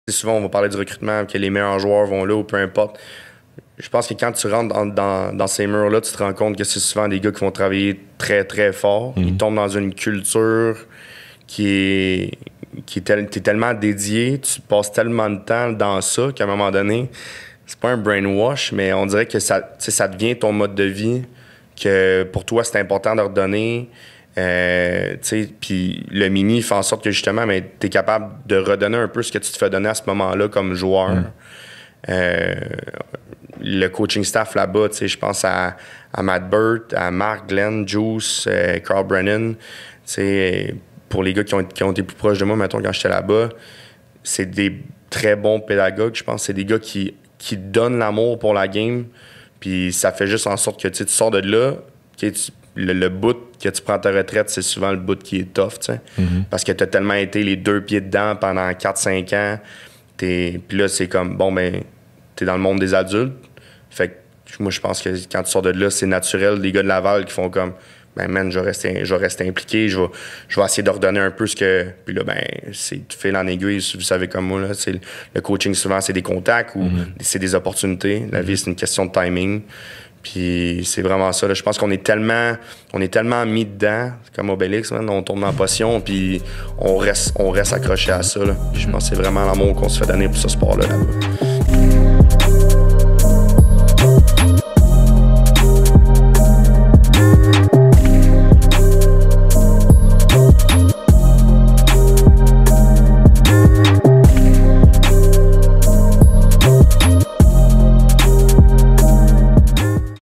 PFP Clips | La culture du Rouge&Or
Jean-Philippe Bégin, entraîneur-chef des Géants du Cégep Saint-Jean-sur-Richelieu, nous explique ce qui selon lui est spécial avec la culture du Rouge et Or de l'Université Laval.

Pour recevoir jusqu’à 95$ de rabais sur votre première réservation avec Airbnb
*** Merci à tous ceux qui nous supportent! ***

Design par Chien Champion